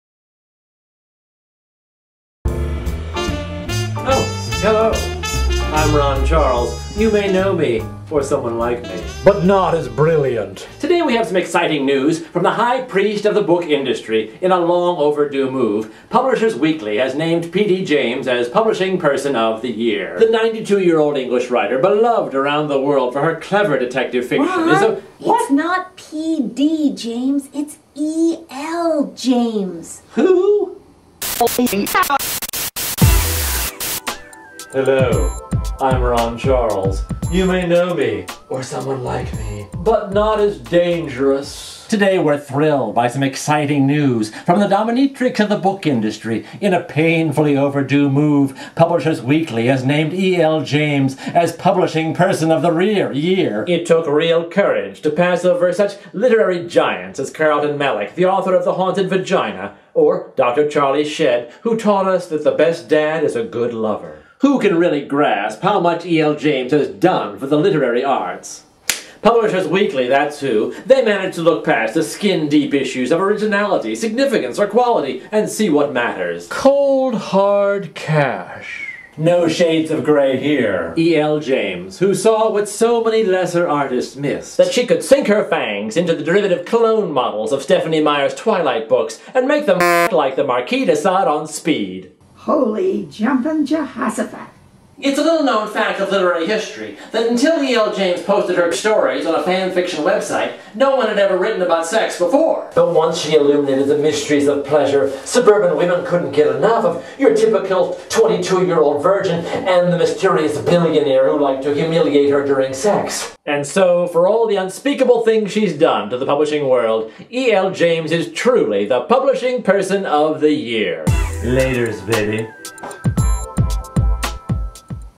Fifty Shades of Grey, EL James is Publishing Person of the Year
E.L. James, "Fifty Shades of Grey," has been named the Publishing Person of the Year by Publishers Weekly. The Washington Post's Ron Charles, celebrates. For more Totally Hip Video Book Reviews go